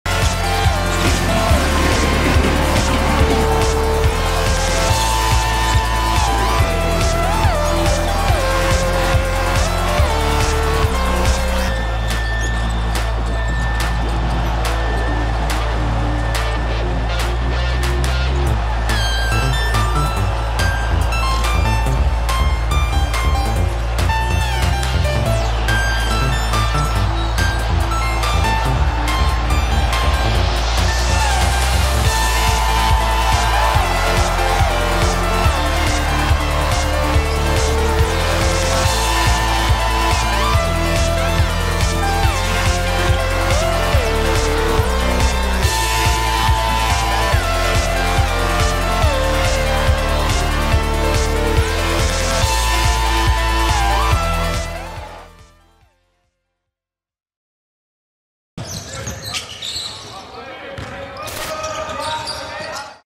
La Supercopa Endesa, a toda velocidad | Supercopa Endesa 2024
Os dejamos con las mejores jugadas Renfe de la Supercopa Endesa. La velocidad de Kevin Punter y Facu Campazzo, los contraataques colectivos de UCAM Murcia y la rapidez de Kendrick Perry son dignas de ser nombradas jugadas de alta velocidad.